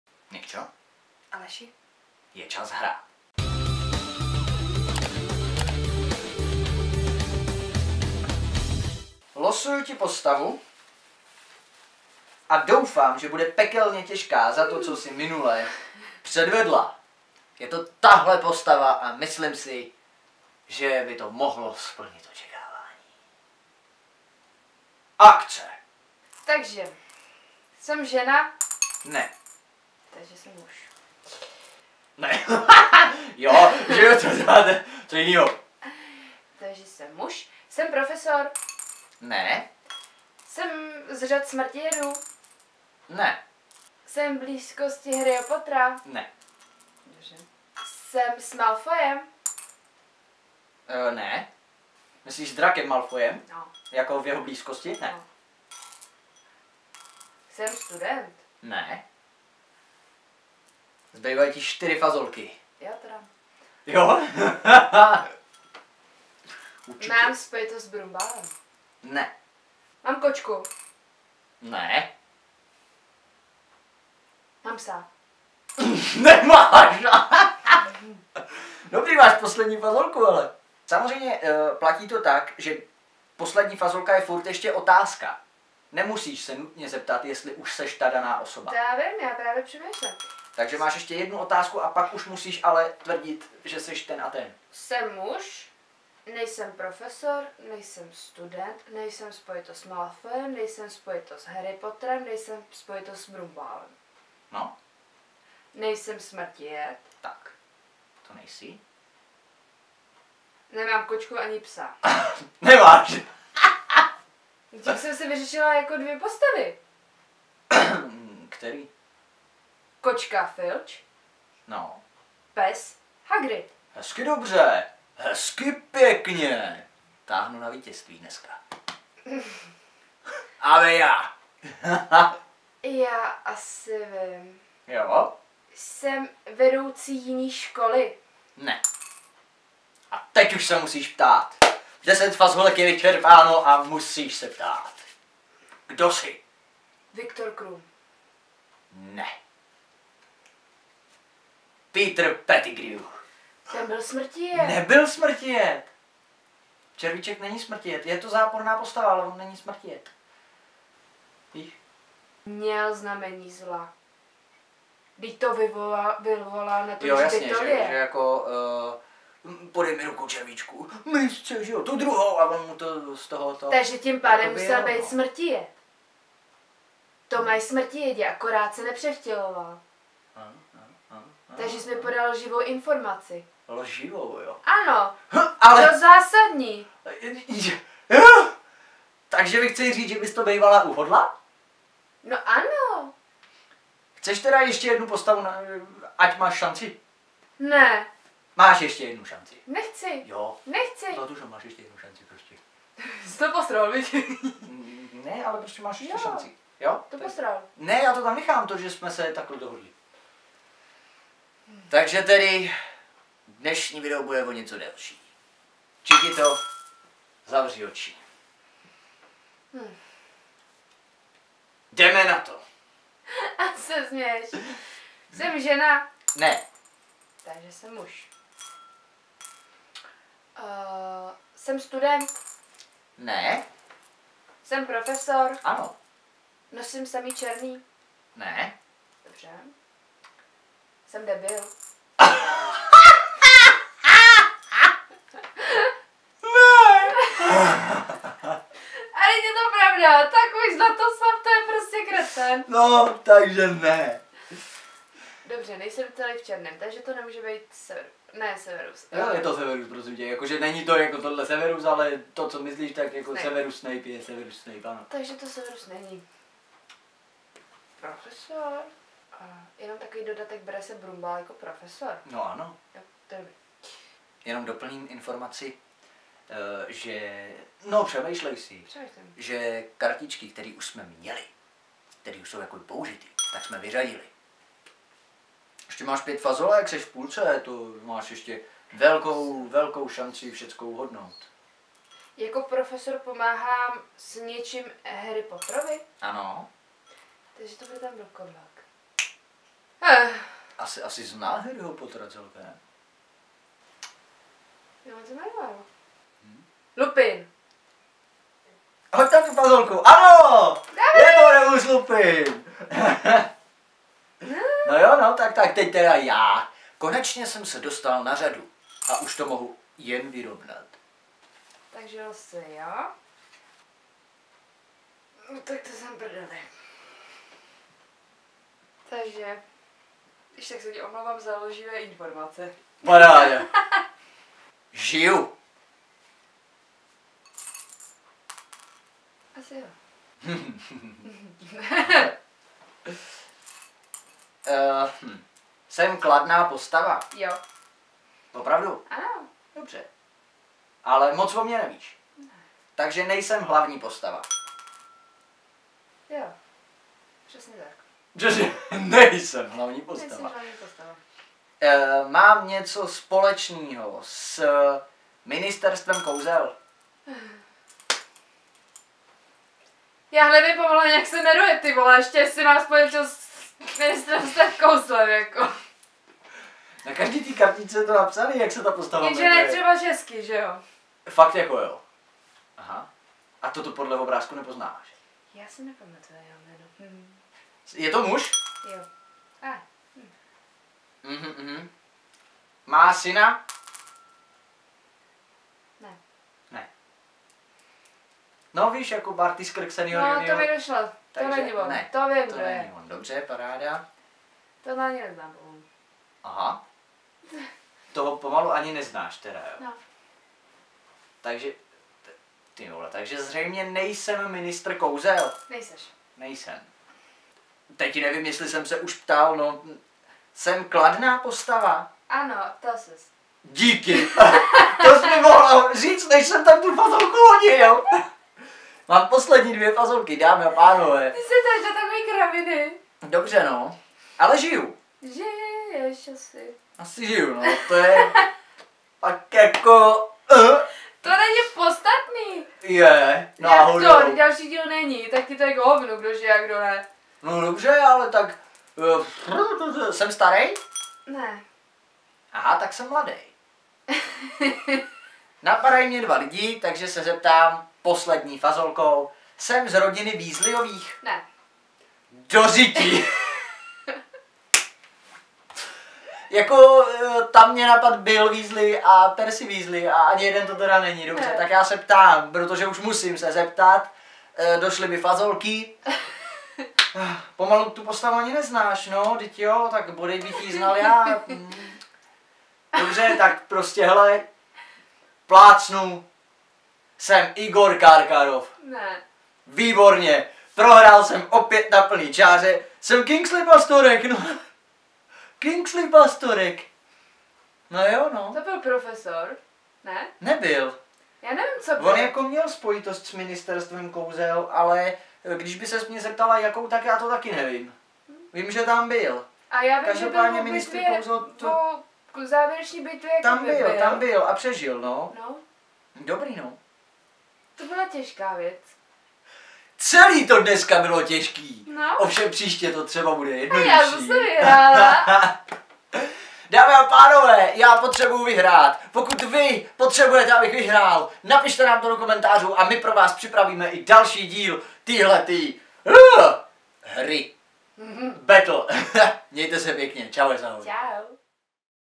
HÁDEJ, KDO JSEM Z HARRYHO POTTERA #3 - NEFÉR PLAY
"Hádej, kdo jsem" ve stylu Harryho Pottera se na mém kanále celkem uchytilo🤩 Myslel jsem si, že jako fanda této filmové ságy budu celkem zdatný v hádání postav 🥳 Odveta ale nevyšla a Čikita mě porazila na plné čáře. 😍 Dneska je řada na mě! Držte mi palce a zároveň omluvte mou...nefér play. 🎭

- Pokud vidíš tohle: 😍 jsi opravdový fanoušek, co čte popisky! Přiznej to, hoď mi tento smajlík do komentáře.